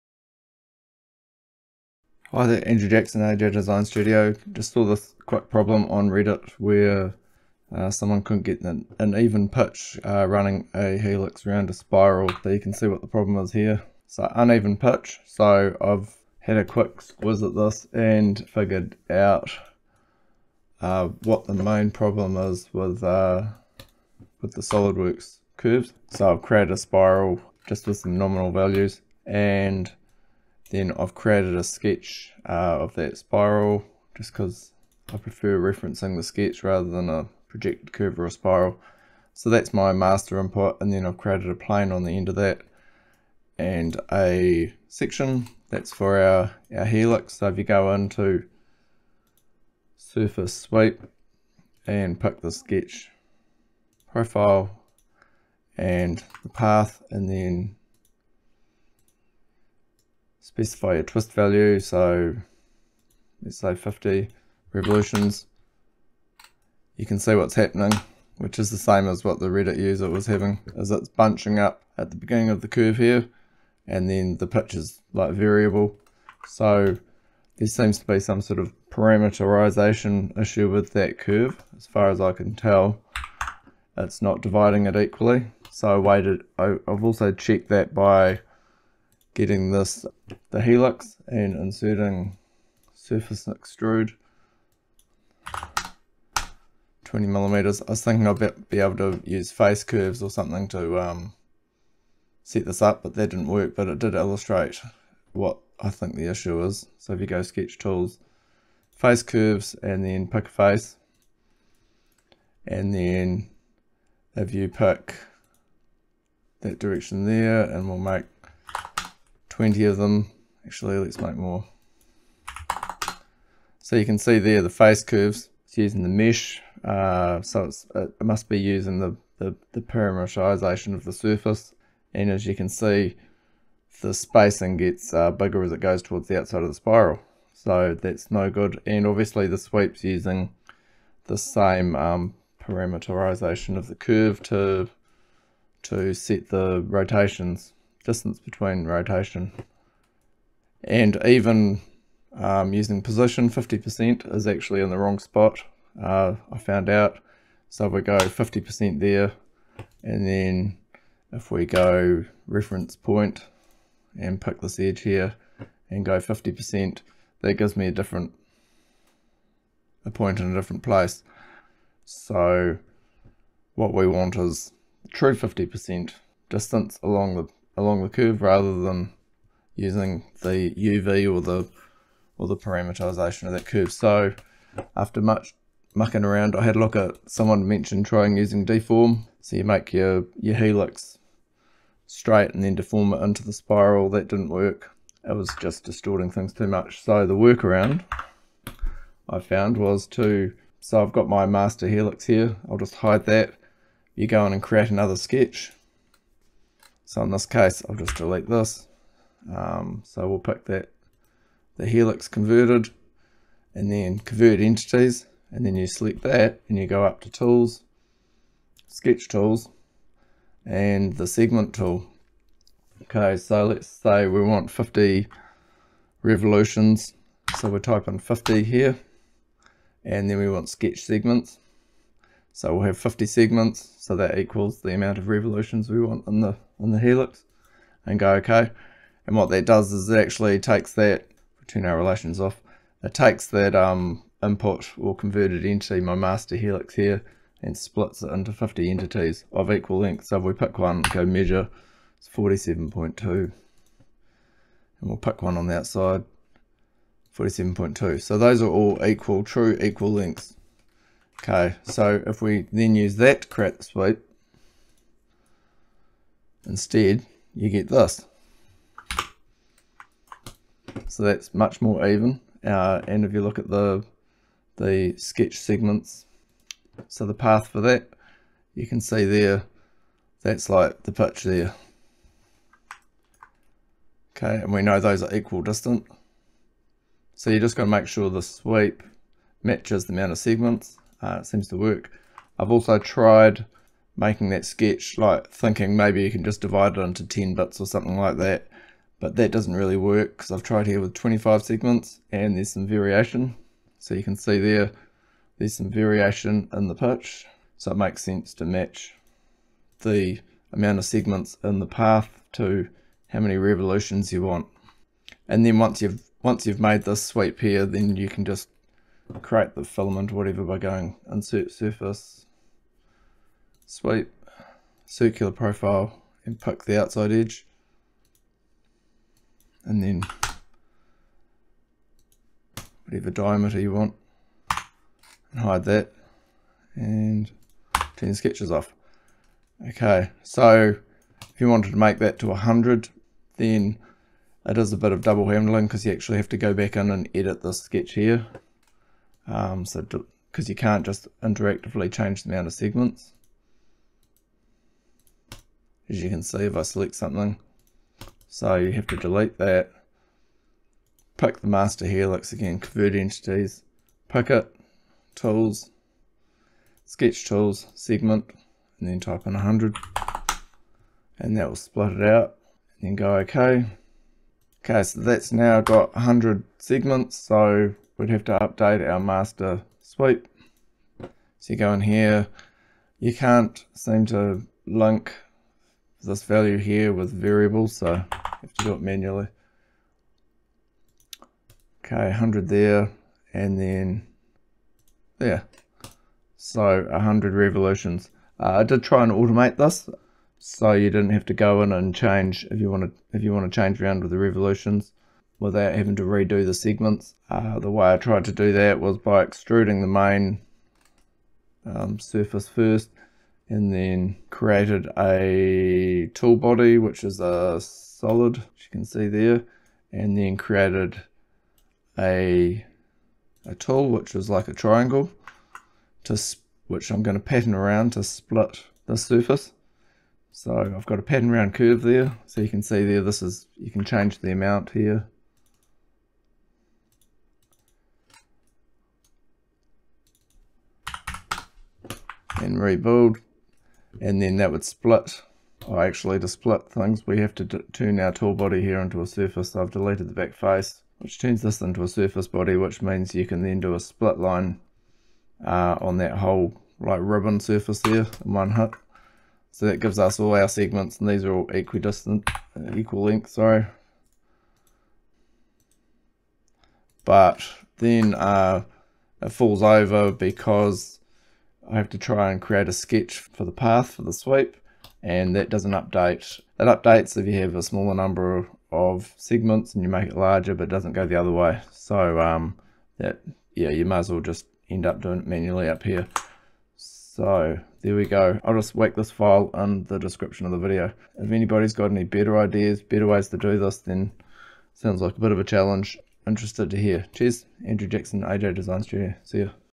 Solidworks Equal Pitch Spiral Helix Problem
This problem was posted on Reddit, where Solidworks will not create a swept helix with an equal pitch between revolutions, when a helix is created around a spiral. I've also tried creating a swept helix along a spline and it has the same issue. This video covers one way around the problem, which is to split the sweep path into equal length segments, using the sketch segment tool.